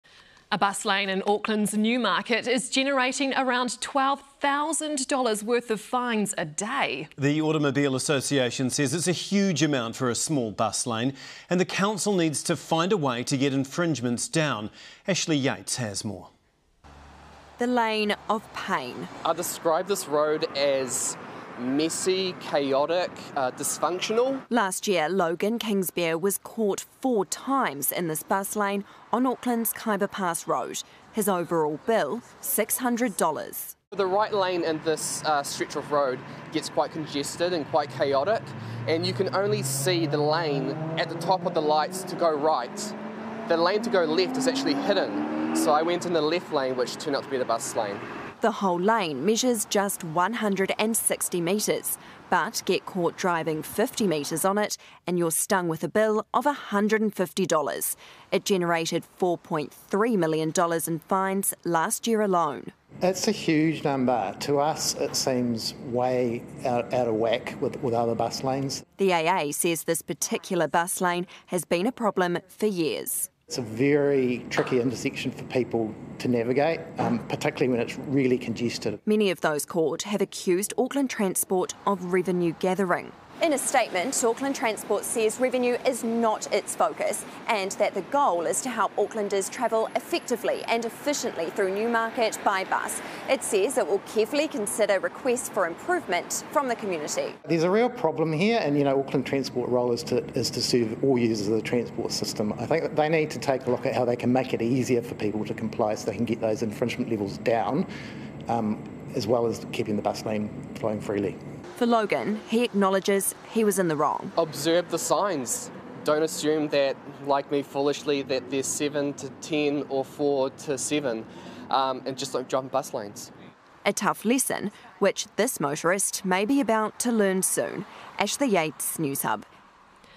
Snapped: Auckland Transport making $4.3m a year from ONE traffic camera's fines | Newshub
In the lane of pain, Auckland road users are being fined an enormous $12,000 a day from ONE single traffic camera - that's a whopping $4.3 million a year. AA says it's "out of whack" as Auckland Transport faces accusations of revenue gathering from drivers caught out. Ashleigh Yates reports.